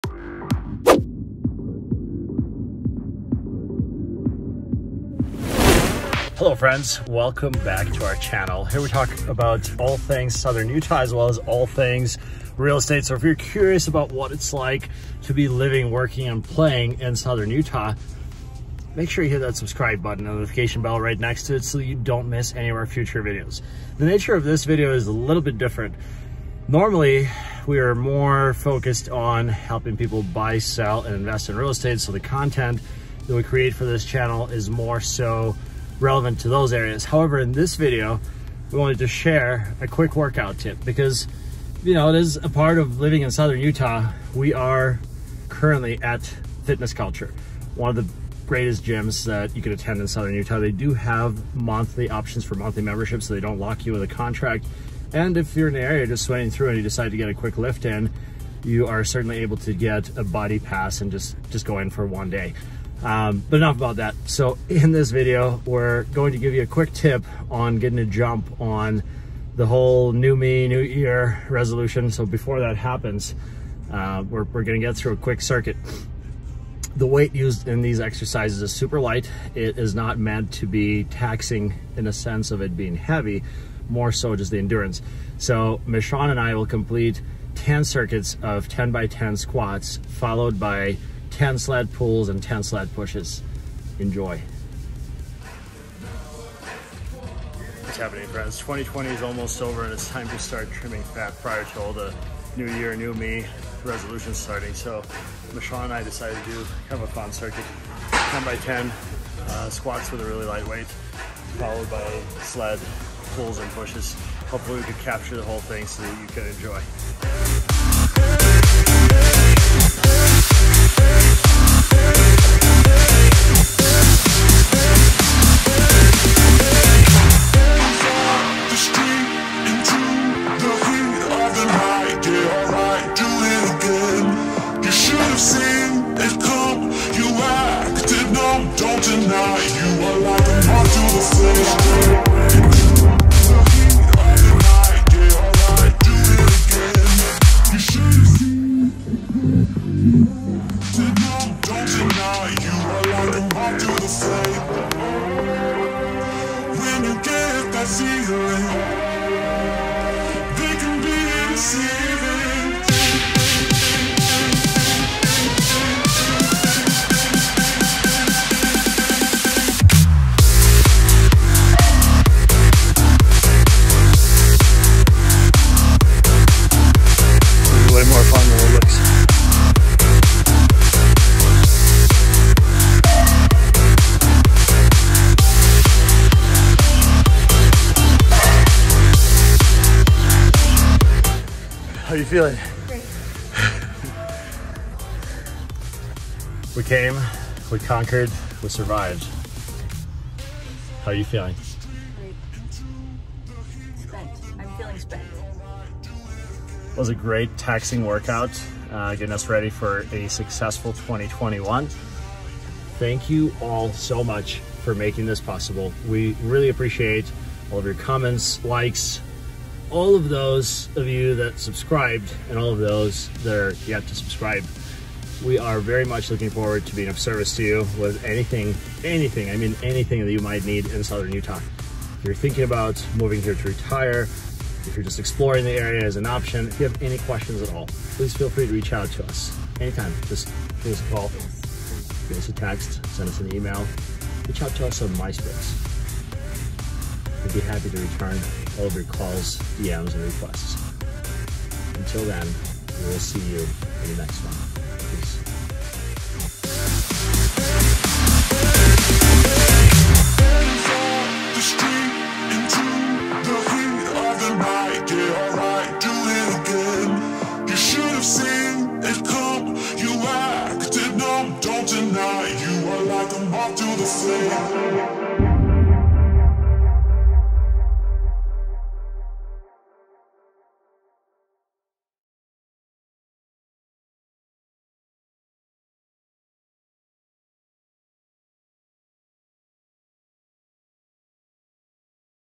Moving to Southern Utah |Best places to exercise
If you are moving to Southern Utah and a large part of your new year's resolution is staying in shape - this video is for you!
In this video, we will cover our favorite place to exercise in St George, and share a great tip for a fat-melting workout that switches things up a bit.

Youtube Channel Standard Description

🎦Watch videos about best places to live in Southern Utah